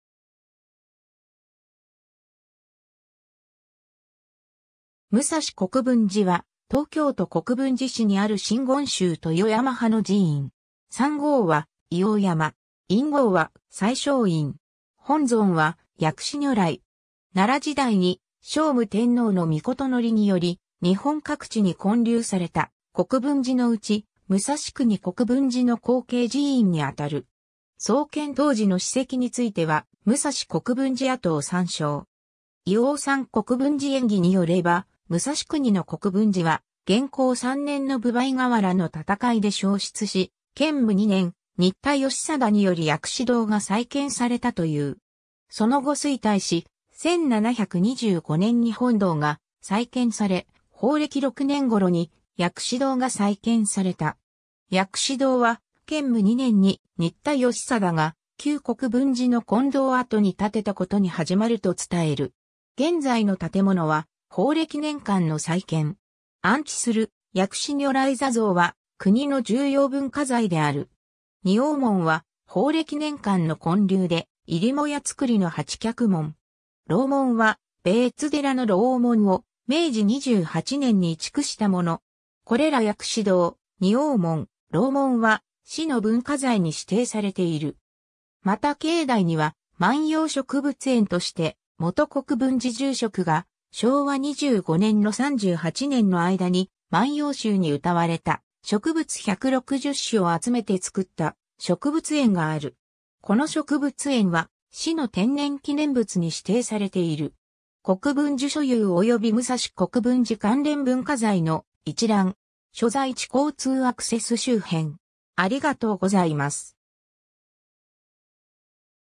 武蔵国分寺
武蔵国分寺（むさしこくぶんじ）は、東京都国分寺市にある真言宗豊山派の寺院。
山号は医王山。
院号は最勝院。
 奈良時代に聖武天皇の詔により日本各地に建立された国分寺のうち、武蔵国国分寺の後継寺院にあたる。
創建当時の史跡については武蔵国分寺跡を参照。
 『医王山国分寺縁起』によれば、武蔵国の国分寺は、元弘3年（1333年）の分倍河原の戦いで焼失し、建武2年（1335年）、新田義貞により薬師堂が再建されたという。
その後衰退し、1725年に本堂が再建され、宝暦6年（1756年）頃に薬師堂が再建された。
 薬師堂は、建武2年（1335年）に新田義貞が旧国分寺の金堂跡に建てたことに始まると伝える。
現在の建物は宝暦年間（1751年-1763年）の再建。
安置する薬師如来坐像は国の重要文化財である。
仁王門は宝暦年間（1751年-1763年）の建立で、入母屋造の八脚門。
楼門は米津寺（東久留米市）の楼門を明治28年に移築したもの。
これら薬師堂・仁王門・楼門は、市の文化財に指定されている。
 また境内には「万葉植物園」として、元国分寺住職が昭和25年-38年（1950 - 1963年）の間に万葉集に歌われた植物160種を集めて造った植物園がある。
この植物園は市の天然記念物に指定されている。
 国分寺所有および武蔵国分寺関連文化財の一覧。
 所在地 交通アクセス 周辺